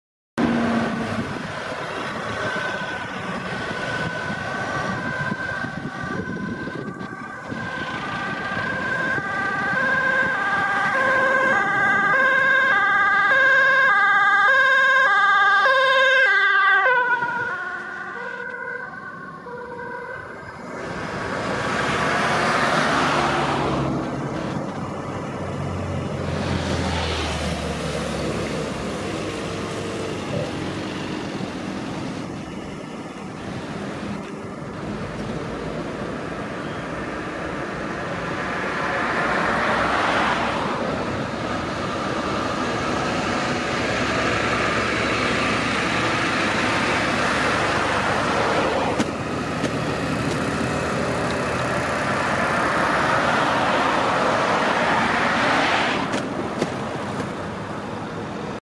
A1 Ambulance 21 114 (Kan er lastig door) met spoed naar melding in Grave
112NieuwsSchaijk112
-bedankt dat jullie hebben gekeken naar deze video! - wil je nog meer zien? -laat een like achter!